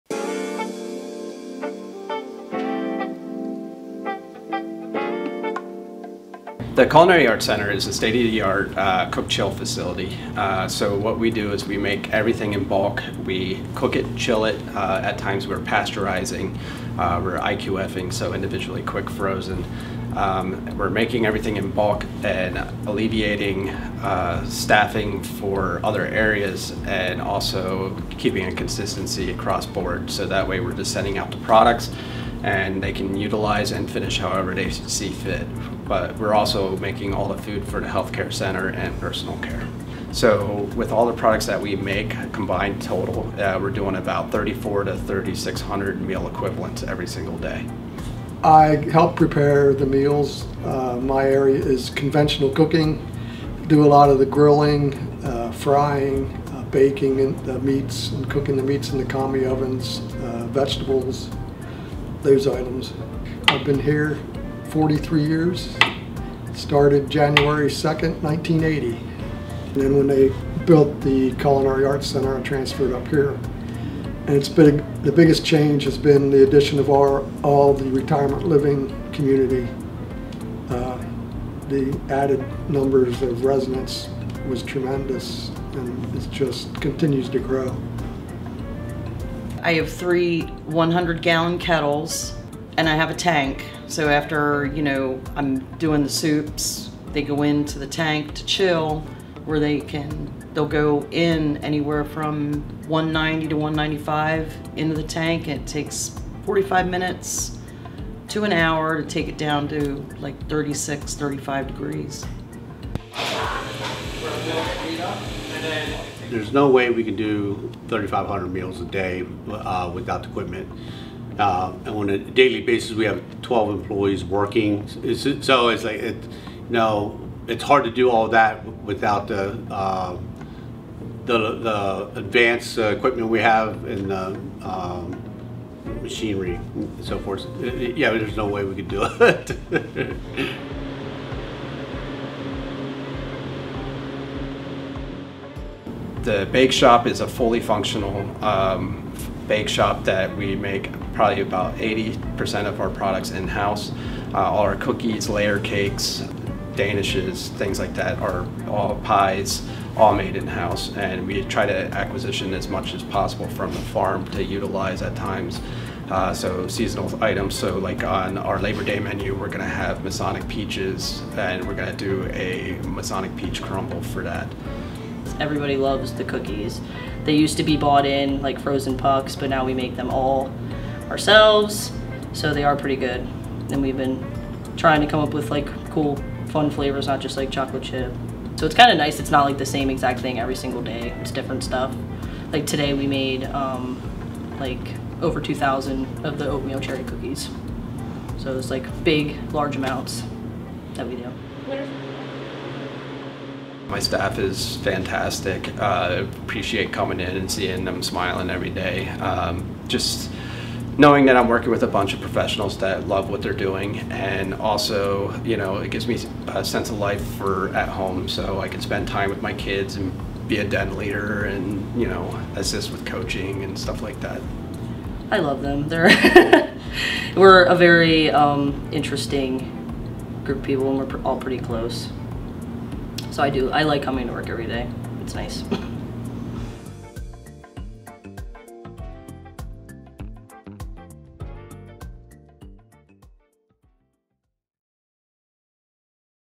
Celebrating Culinary Arts
Masonic Village at Elizabethtown's culinary arts center uses state-of-the-art technology to provide thousands of meals and food daily to feed residents and employees across campus. Staff use their skills and creativity, as well as fresh produce and beef grown on site, to delight diners.